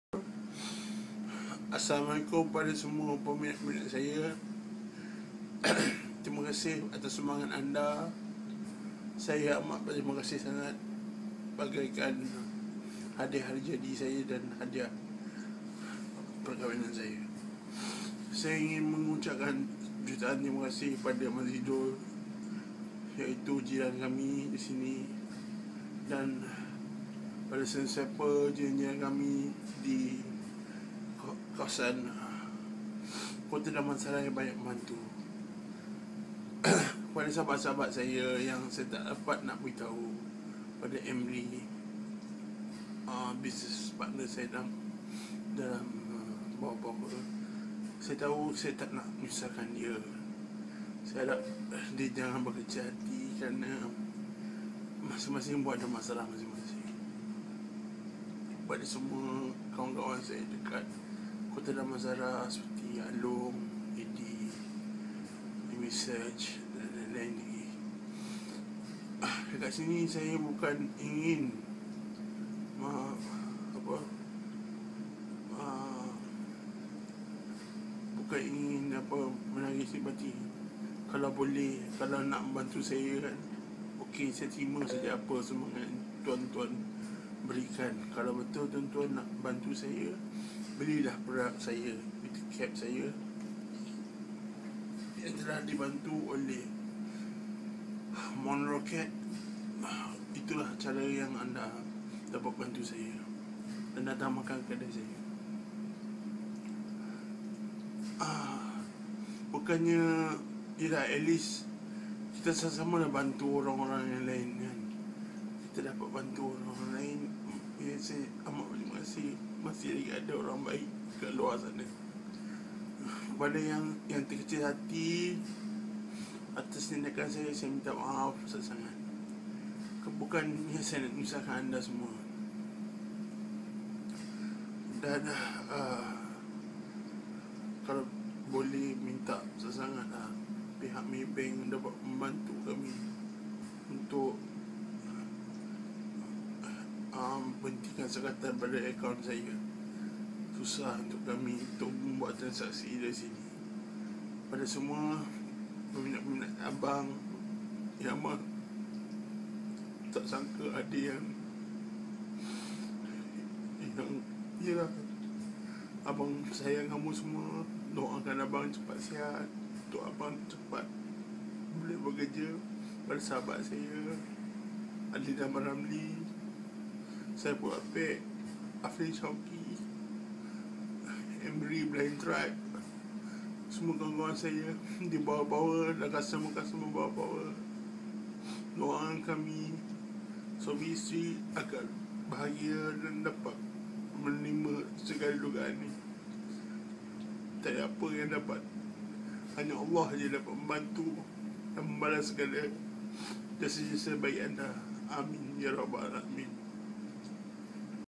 Al-Fatihah Buat Bob Kuman | Semoga Arwah Berada Di Kalangan Orang Yang Beriman
Ucapan terima kasih arwah bob kuman @ Husni Mohd Rawi kepada semua yang telah membantu beliau. Ramai-ramai kita sedekahkan al-fatihah buat arwah mudah-mudahan arwah di tempatkan dikalangan orang-orang yang beriman insyallah. Arwah disahkan meninggal dunia oleh pihak kakitangan Hospital Sungai Buluh pada jam 4.38 petang tadi. 3/5/2012